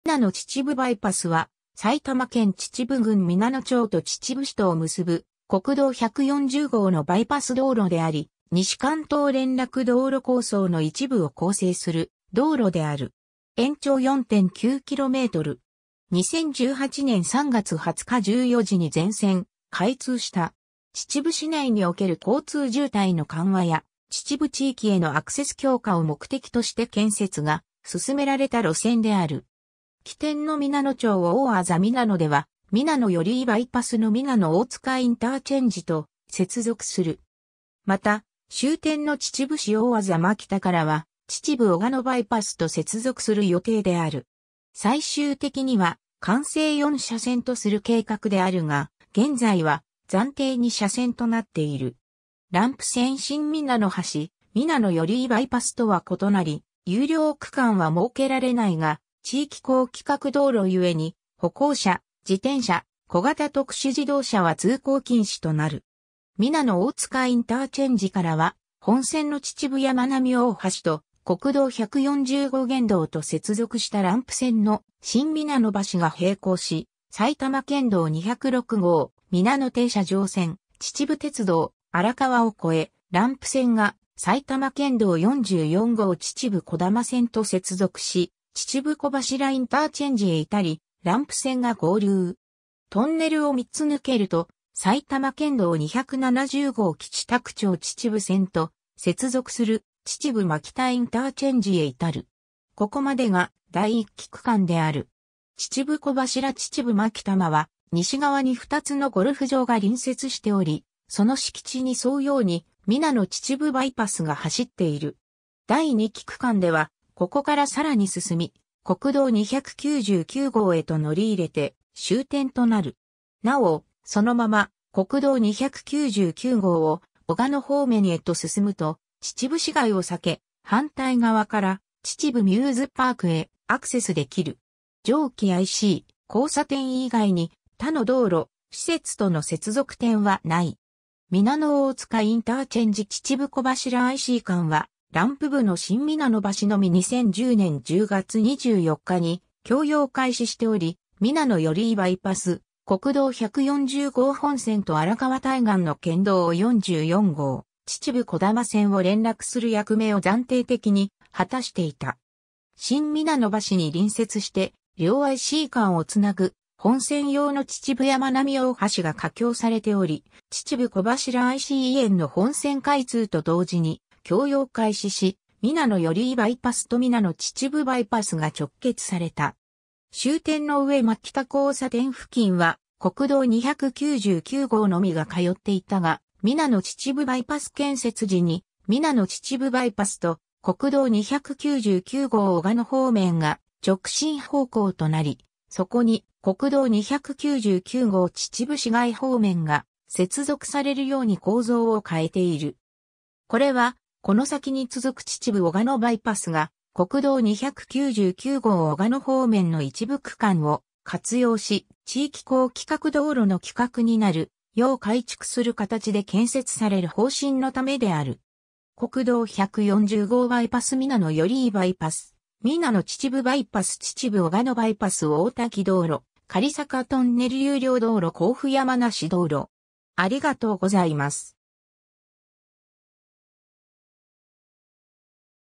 皆野秩父バイパス
皆野秩父バイパス（みなのちちぶバイパス）は、埼玉県秩父郡皆野町と秩父市とを結ぶ国道140号のバイパス道路であり、西関東連絡道路（地域高規格道路）構想の一部を構成する道路である。
延長4.9km。
2018年（平成30年）3月20日14時に全線開通した。
 秩父市内における交通渋滞の緩和や秩父地域へのアクセス強化を目的として建設が進められた路線である。
起点の皆野町大字皆野では皆野寄居バイパスの皆野大塚インターチェンジと接続する。
また、終点の秩父市大字蒔田からは秩父小鹿野バイパスと接続する予定である。
 最終的には完成4車線とする計画であるが、現在は暫定2車線となっている。
 ランプ線新皆野橋 （現在は手前側に本線の秩父やまなみ大橋が架かる） 皆野寄居バイパスとは異なり、有料区間は設けられないが、地域高規格道路ゆえに、歩行者・自転車・小型特殊自動車は通行禁止となる。
 皆野大塚インターチェンジからは、本線の秩父やまなみ大橋と国道140号現道と接続したランプ線の新皆野橋が平行し、埼玉県道206号皆野停車場線（接続なし）、秩父鉄道、荒川を超え、ランプ線が埼玉県道44号秩父児玉線と接続し、秩父小柱インターチェンジへ至り、ランプ線が合流。
 トンネルを3つ抜けると、埼玉県道270号吉田久長秩父線と接続する秩父蒔田インターチェンジへ至る。
ここまでが第1期区間である。
秩父小柱 - 秩父蒔田間は、西側に2つのゴルフ場が隣接しており、その敷地に沿うように皆野秩父バイパスが走っている（ゴルフ場への接続は無い）。
 第2期区間では、ここから更に進み、国道299号（秩父小鹿野バイパス予定地）へと乗り入れて終点となる。
 なお、そのまま国道299号を小鹿野方面へと進むと、秩父市街を避け、反対側から秩父ミューズパークへアクセスできる（当面国道140号現道への接続がないことから、当バイパスの整備効果としては関越自動車道花園インターチェンジから皆野寄居バイパス及び当バイパスを経由することによるミューズパークへのアクセス時間短縮を謳っている）。
 上記IC・交差点以外に、他の道路・施設との接続点は無い。
 皆野大塚IC - 秩父小柱IC間はランプ部の新皆野橋（489m）のみ2010年10月24日に供用開始しており、皆野寄居バイパス・国道140号本線と荒川対岸の県道44号秩父児玉線を連絡する役目を暫定的に果たしていた。
 新皆野橋に隣接して、両IC間を繋ぐ本線用の秩父やまなみ大橋（725.5 m）が架橋されており、秩父小柱IC以遠の本線開通と同時に供用開始し、皆野寄居バイパスと皆野秩父バイパスが直結された。
 終点の上蒔田交差点付近は、国道299号のみが通っていたが、皆野秩父バイパス建設時に、皆野秩父バイパスと国道299号小鹿野方面が直進方向となり、そこに国道299号秩父市街方面が接続されるように構造を変えている。
これは、この先に続く秩父小鹿野バイパスが、国道299号小鹿野方面の一部区間を活用し、地域高規格道路の規格になるよう改築する形で建設される方針のためである。
 （熊谷方面）国道140号バイパス - 皆野寄居バイパス - 皆野秩父バイパス - 秩父小鹿野バイパス - 大滝道路 - 雁坂トンネル有料道路 - 甲府山梨道路（甲府方面）